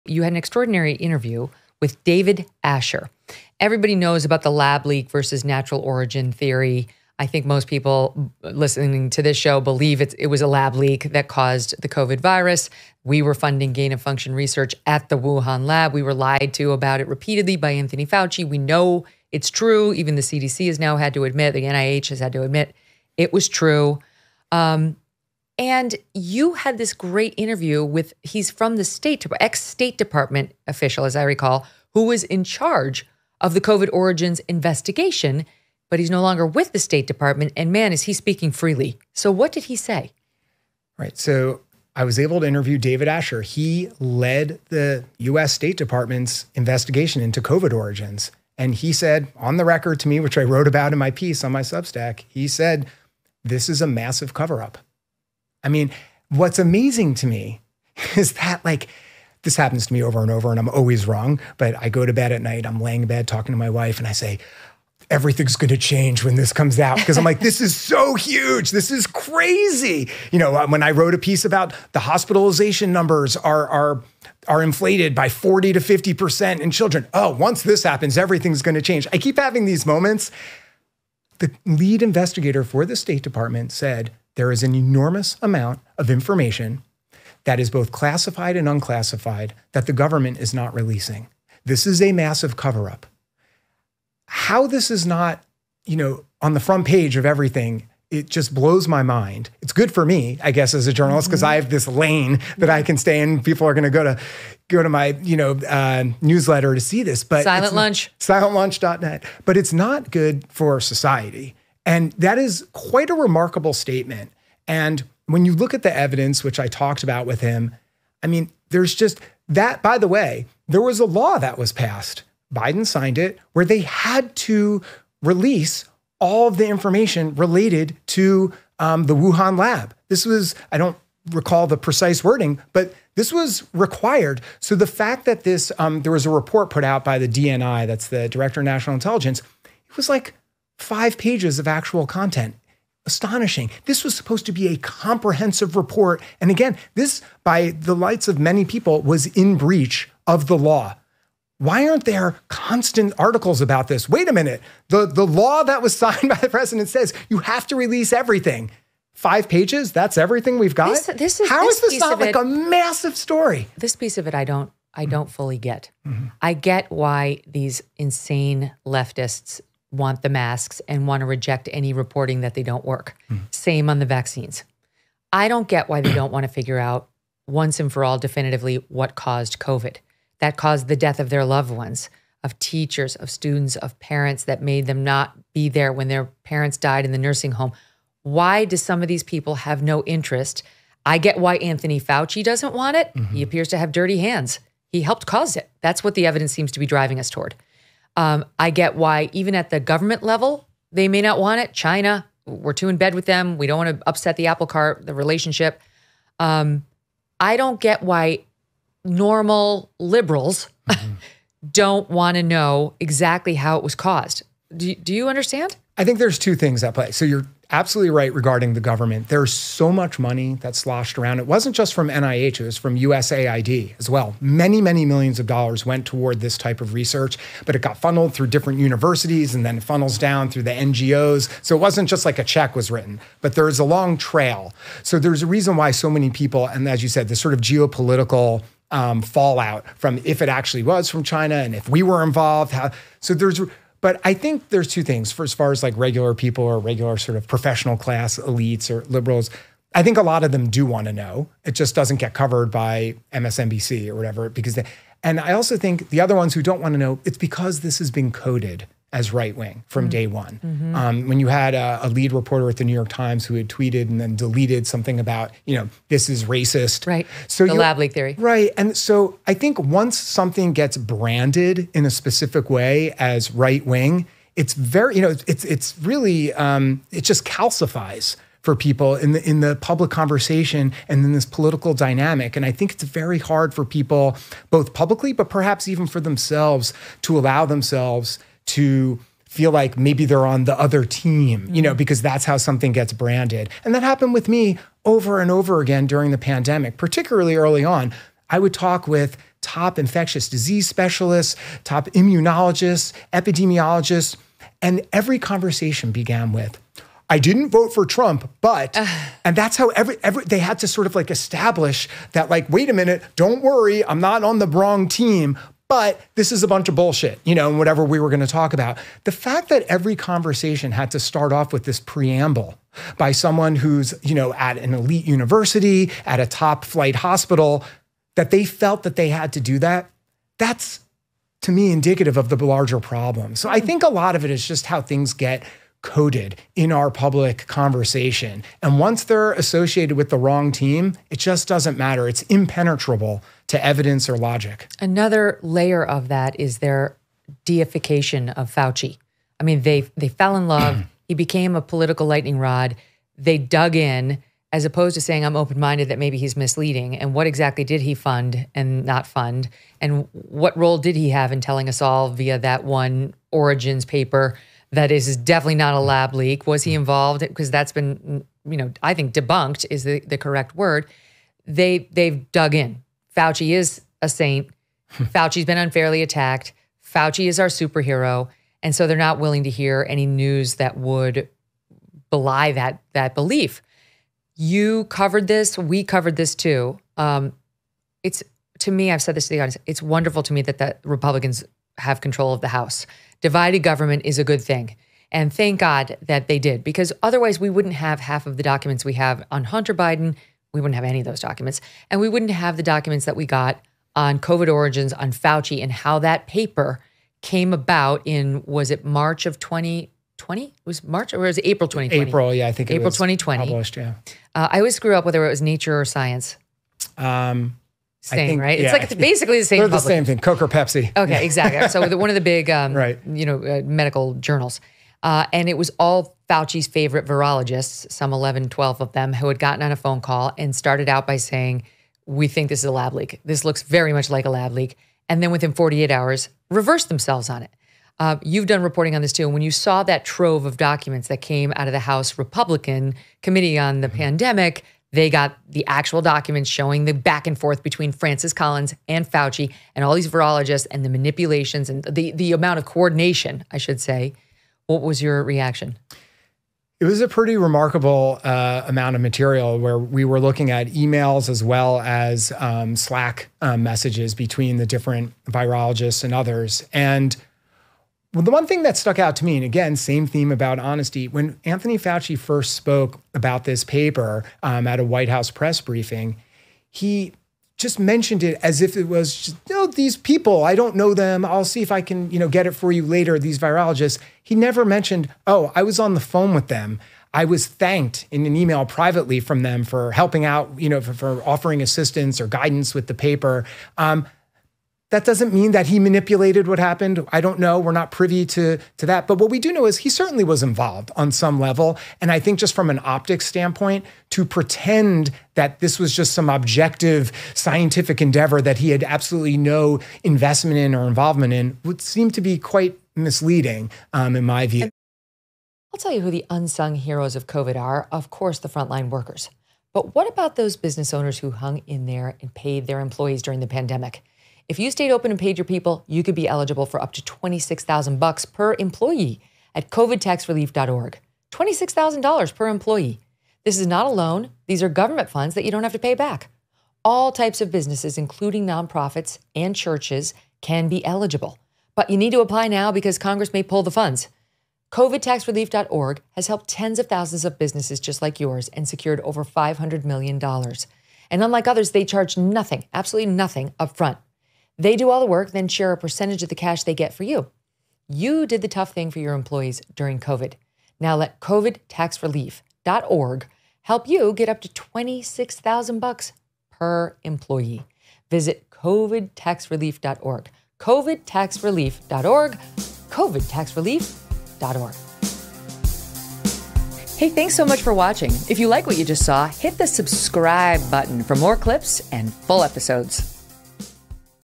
"Cover Up" About COVID Origins Revealed... But Why Won't the Media Cover It? With David Zweig
Megyn Kelly is joined by David Zweig, journalist for The Free Press and the Silent Lunch Substack, to discuss his interview with a former State Department official who reveals a "cover-up" on COVID origins, whether the government is hiding information about COVID origins, why the media is ignoring this story, and more.